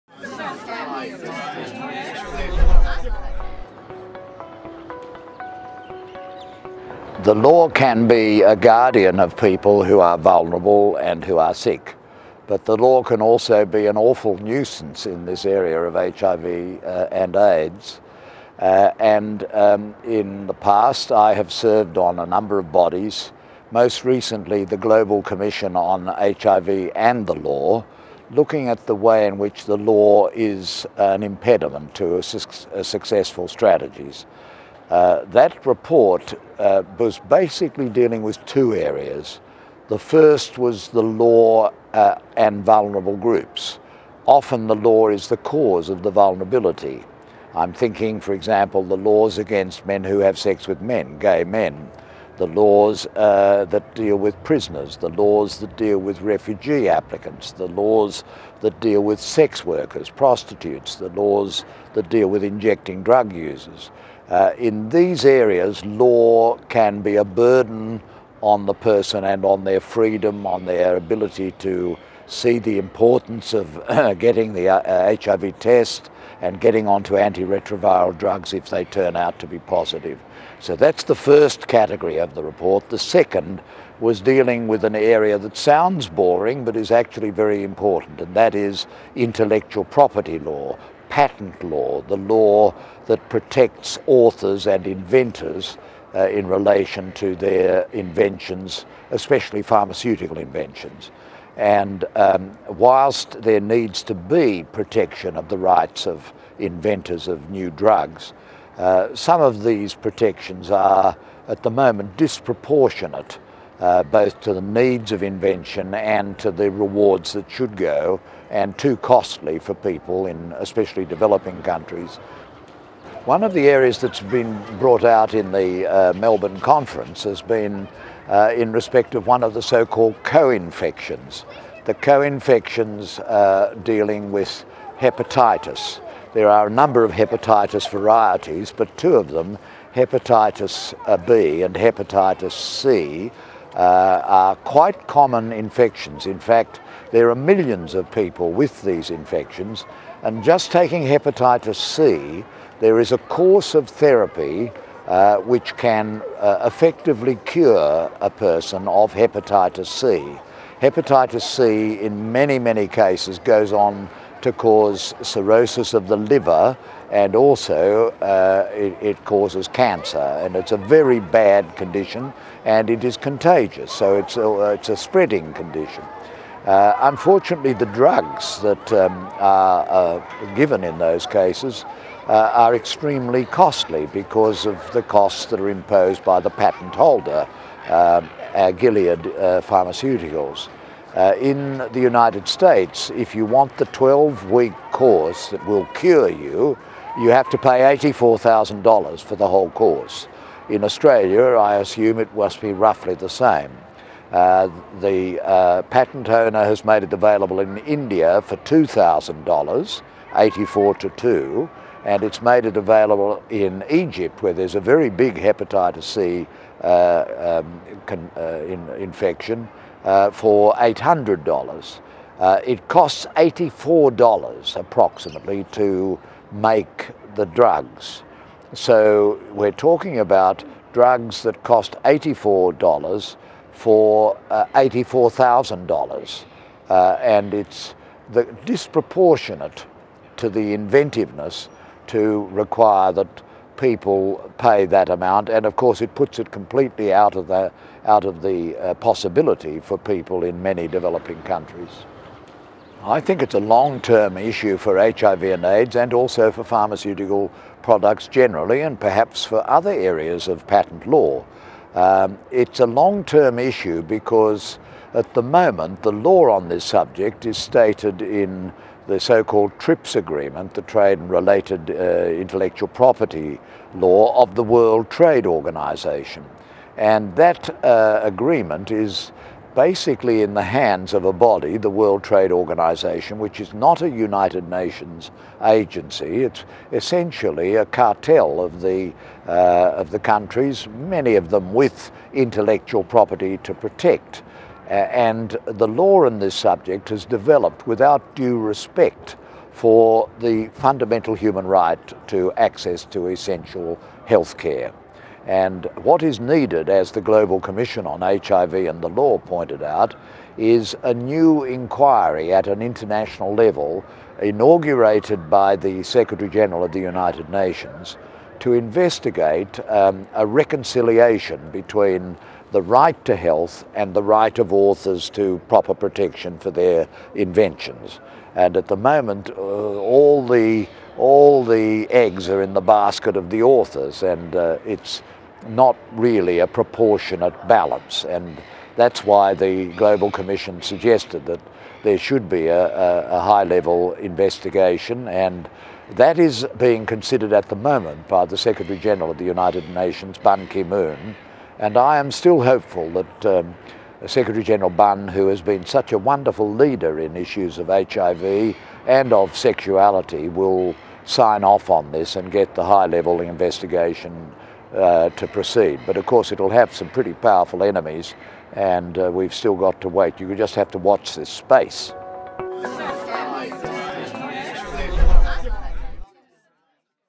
Michael Kirby on how the law impacts HIV
The Hon. Michael Kirby, visiting professorial fellow at UNSW Australia, talk about how the law impacts HIV